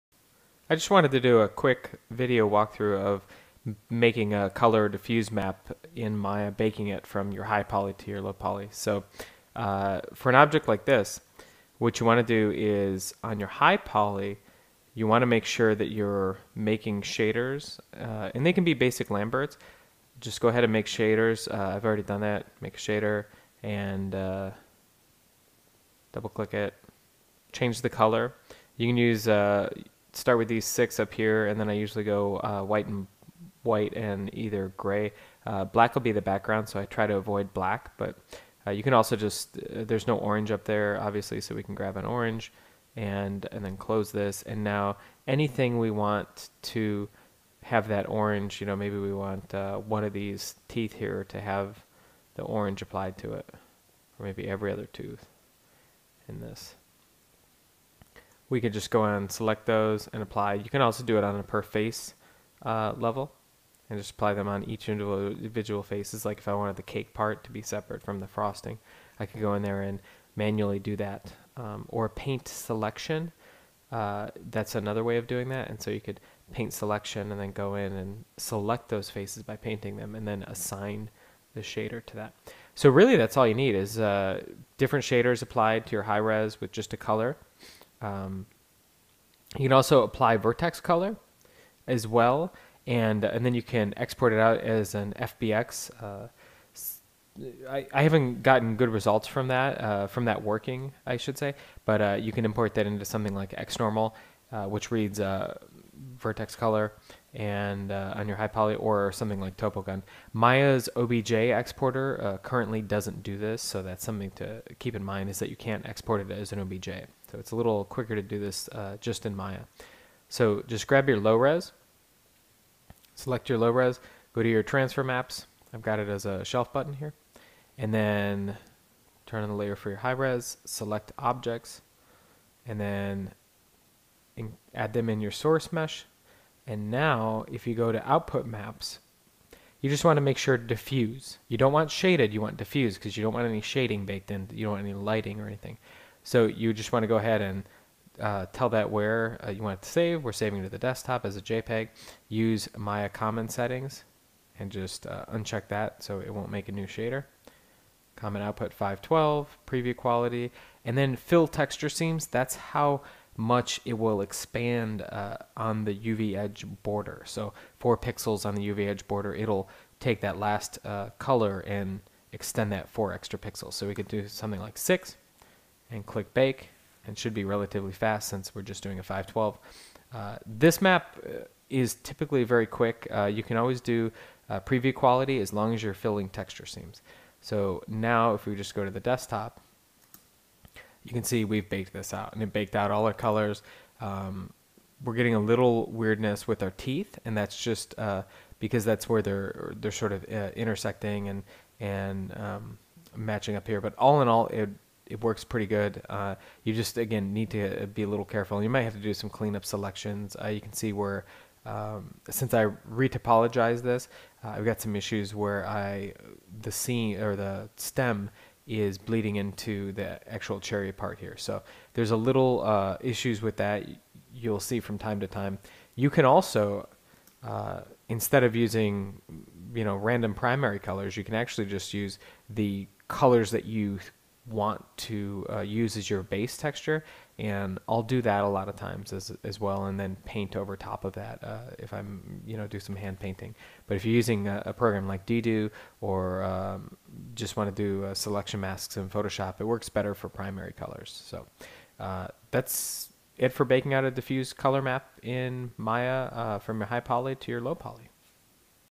Baking color (diffuse) maps from high poly to low poly in Maya
This quick video covers baking diffuse or color maps from your high polygon mesh in Maya to your UV mapped, low resolution model in Maya for use with texturing for game art creation.